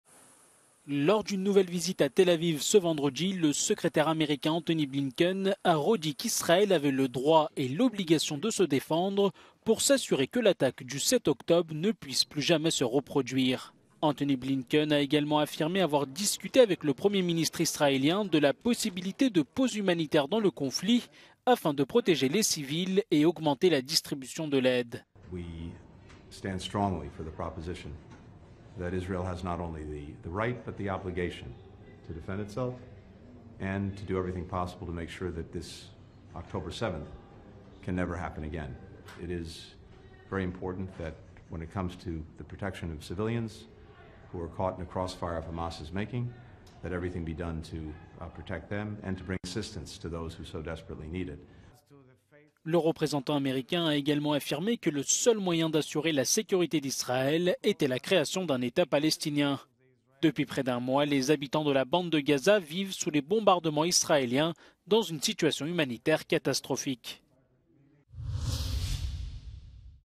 Blinken à Tel-Aviv: Israël a "le droit" et "l'obligation" de "se défendre"
Le secrétaire d'Etat américain Antony Blinken a redit vendredi, lors d'une nouvelle visite à Tel-Aviv, qu'Israël avait "le droit" et "l'obligation" de se "défendre" pour s'assurer que l'attaque du 7 octobre ne puisse "plus jamais se reproduire".